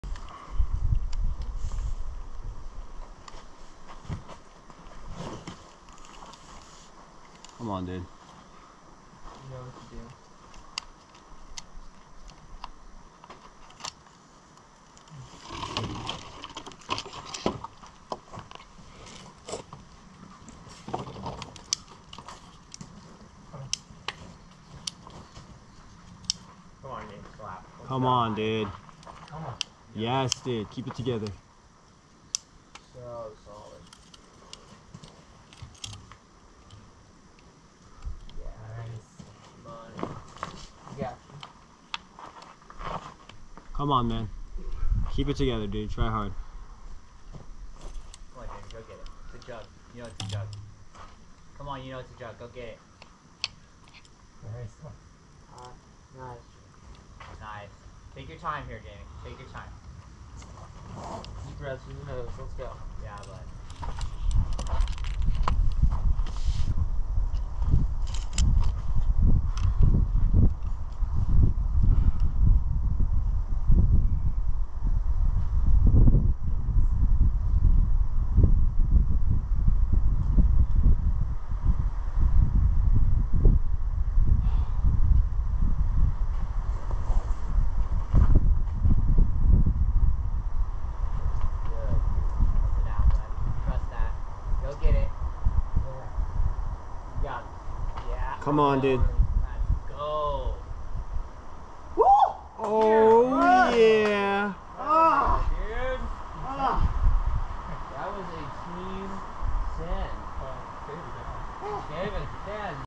Gluttony (V8), Palm Springs Aerial Tramway
My camera lens somehow caught some steam when it was recording, making it a bit blurry